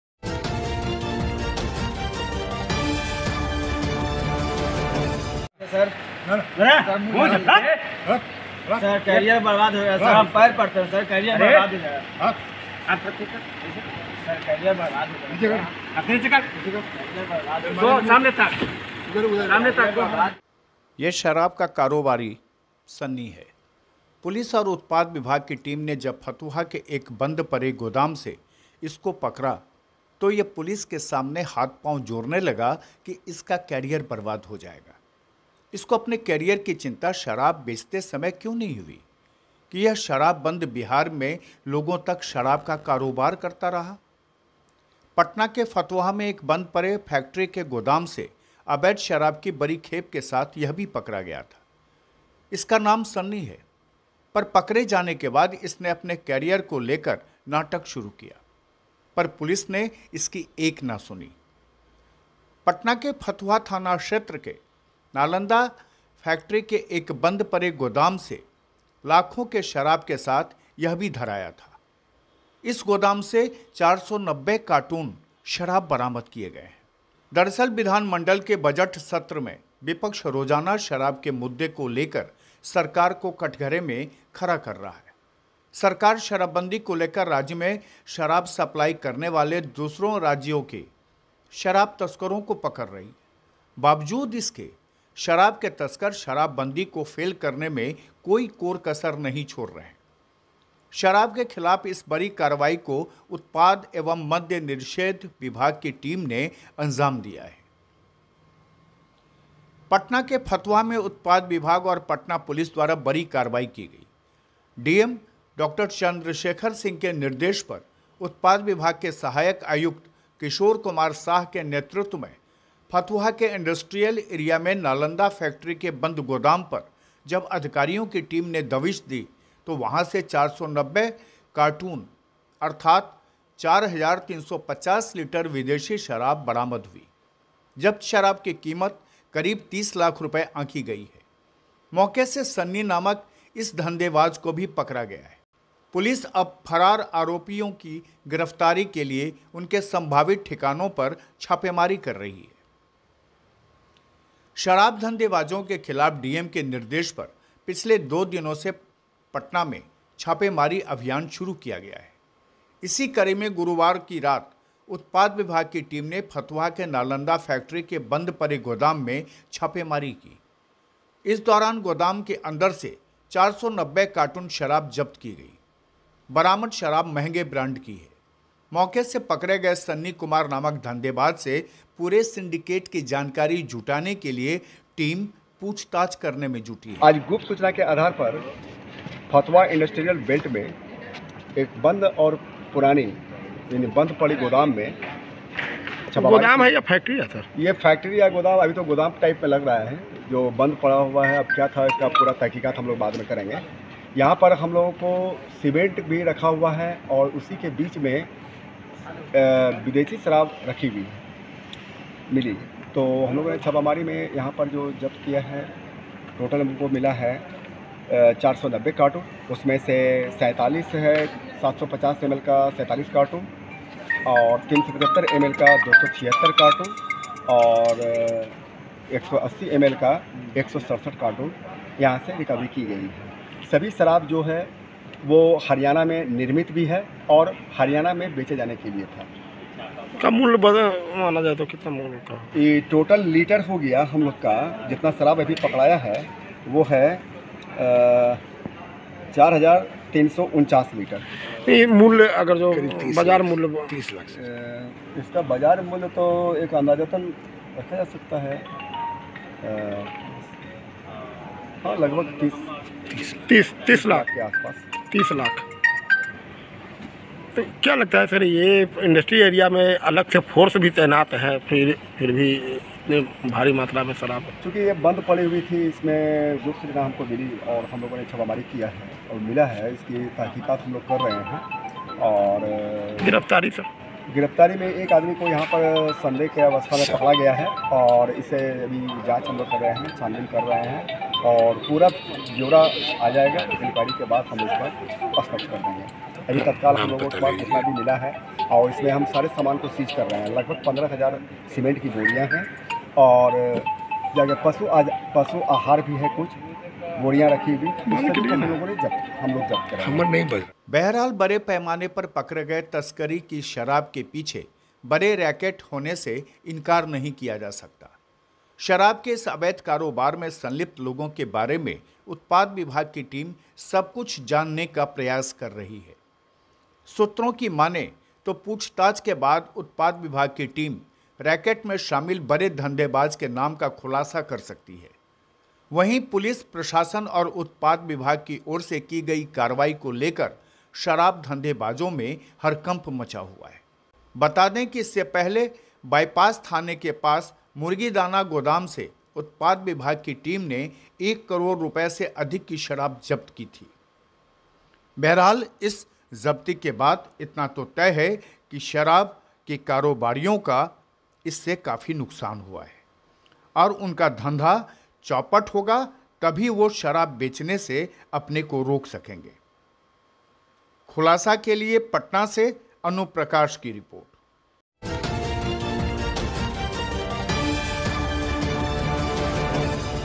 शराब कारोबारी पर छापा : तस्कर पुलिस को देखते ही गिड़गिड़ाया कैरियर खराब हो जाएगा !
पटना से अनु प्रकाश की रिपोर्ट

यह शराब का धन्धेवाज़ सन्नी है ... पुलिस और उत्पाद विभाग की टीम ने जब फतुहा के एक बंद पड़े गोदाम से इसको पकड़ा तो यह पुलिस के सामने हाथ पांव जोड़ने लगा की इसका कैरियर बर्बाद हो जाएगा ... इसको अपने कैरियर की चिंता शराब बेचते समय क्यो नहीं हुई की यह शराब बंद बिहार में लोंगो तक शराब का कारोबार करता था . पटना के फतुहा में  एक बंद पड़े फैक्ट्री के गोदाम से अवैध शराब की बड़ी खेप के साथ यह भी पकड़ा गया था . इसका नाम सन्नी है . पर पकडे जाने के बाद इसने अपने कैरियर को लेकर नाटक शुरू किया पर पुलिस ने इसकी एक ना सुनी .सन्नी ... पटना के फतुहा थाना क्षेत्र के नालंदा फैक्ट्री के एक बंद पड़े गोदाम से में लाखिओ के शराब के साथ धराया था .इस गोदाम से 490 कार्टून शराब बरामद किये गए हैं .
दरसल विधानमंडल के बजट सत्र में विपक्ष रोजाना शराब के मुद्दे को लेकर सरकार को कटघरे में खडा कर रहा है . सरकार शराब बंदी को लेकर राज्य में शराब सप्लाई करने वाले दुसरे राज्यों के शराब तस्करो पकड रही है . बाबजूद इसके शराब के तस्कर शराबबंदी को फेल करने में कोई कोर कसर नहीं छोड़ रहे हैं .शराब के खिलाफ इस बड़ी कारवाई को उत्पाद एवं मद्य निषेध विभाग की टीम ने अंजाम दिया है .
पटना के फतुहा में उत्पाद विभाग और पटना पुलिस द्वारा बड़ी कार्रवाई की गई. डीएम डॉ. चंद्रशेखर सिंह के निर्देश पर उत्पाद विभाग के सहायक आयुक्त किशोर कुमार साह के नेतृत्व में फतुहा के इंडस्ट्रियल एरिया में नालंदा फैक्ट्री के बंद गोदाम पर जब अधिकारियों की टीम ने दबिश दी तो वहां से 490 कार्टून अर्थात 4350 लीटर विदेशी शराब बरामद हुई . जब्त  शराब कीमत करीब 30 लाख रुपये आंकी गयी है. मौके से सन्नी नामक एक धंधेबाज को भी पकड़ा गया है.वही दूसरी घटना में  फतुहा थाने की पुलिस ने पीतांबरपुर फोरलेन के पास पिकअप वैन में लदी हरियाणा निर्मित 20 लाख की शराब पकड़ा है .मौके से धंधेबाजों की एक बाइक भी जब्त की गई है. पुलिस अब फरार आरोपितों की गिरफ्तारी के लिए उनके संभावित ठिकानों पर छापेमारी कर रही है.
शराब धंधेबाजों के खिलाफ डीएम के निर्देश पर पिछले दो दिनों से छापेमारी अभियान शुरू किया गया था .  इसी कड़ी में गुरुवार को उत्पाद विभाग की टीम ने फतुहा के नालंदा फैक्ट्री के बंद पड़े गोदाम में छापेमारी की. इस दौरान गोदाम के अंदर से 490 कार्टून शराब जब्त की गई. बरामद शराब महंगे ब्रांड की है. मौके से पकड़े गये सन्नी कुमार नामक धंधेबाज से पूरे सिंडिकेट की जानकारी जुटाने के लिए टीम पूछताछ करने में जुटी है.
बहरहाल , बड़े पैमाने पर पकडे गए तस्करी की शराब के पीछे बड़े रैकेट होने से इनकार नहीं किया जा सकता. शराब के इस अवैध कारोबार में संलिप्त लोगों के बारे में उत्पाद विभाग की टीम सब कुछ जानने का प्रयास कर रही है.  सूत्रों की मानें तो पूछताछ के बाद उत्पाद विभाग की टीम रैकेट में शामिल बड़े धंधेबाजों के नाम का खुलासा कर सकती है. वहीं पुलिस प्रशासन और उत्पाद विभाग की ओर से की गई कार्रवाई को लेकर शराब धंधेबाजों में हड़कंप मचा हुआ है. बता दें कि इससे पहले बाइपास थाने के पास मुर्गी दाना गोदाम से उत्पाद विभाग की टीम ने एक करोड़ रुपये से अधिक की शराब जब्त की थी.

KHULASA MEDIA
HOUSE NO – AB - 47
SK PURI
BOARING ROAD
PATNA – 800001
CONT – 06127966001